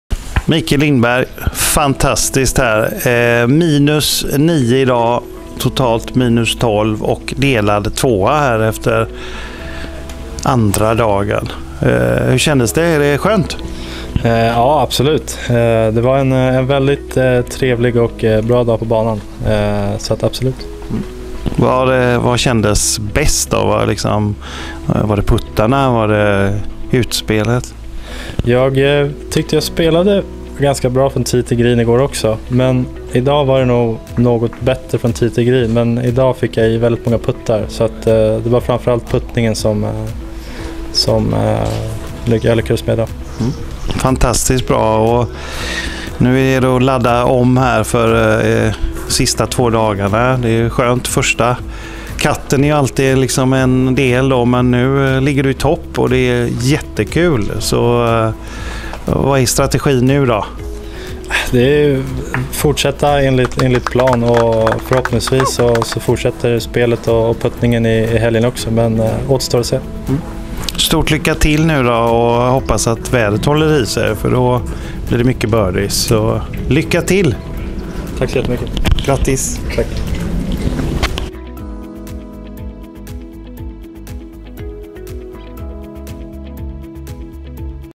Mikael Lindberg sköt 62 slag och banrekord i runda 2 av IGG Challenge 2022
Mikael sköt -9 i sin andra runda och ligger totalt på -12, 1 slag ifrån ledning. Följ upplösningen av tävlingen i helgen på plats på Allerum GK eller Livescoring via webben.